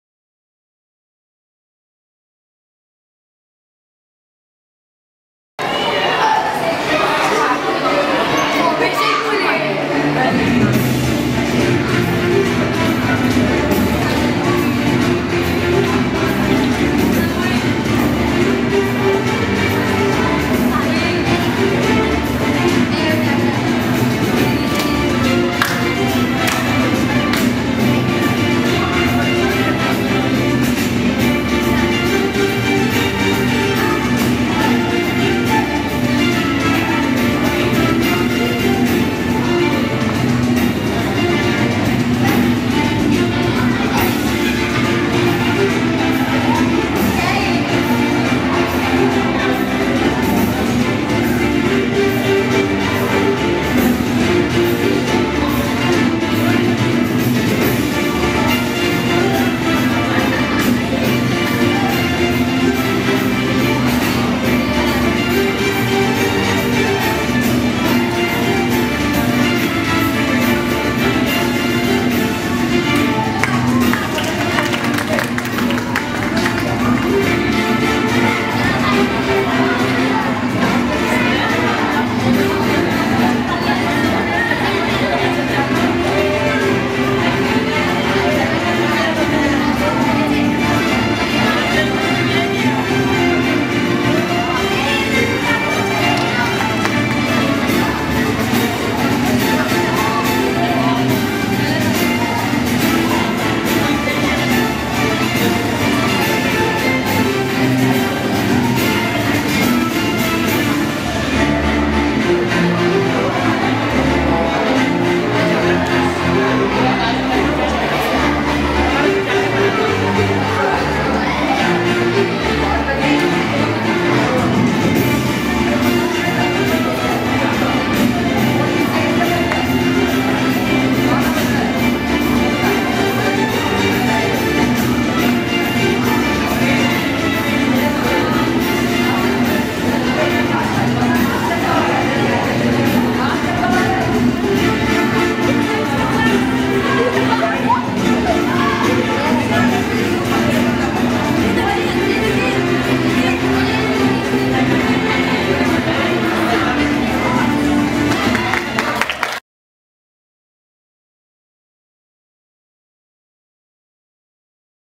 displacement
Gurleen and her team presents displacement reaction efficiently through this dance .
The students proved that you can dance your way through Chemistry and Chemical reactions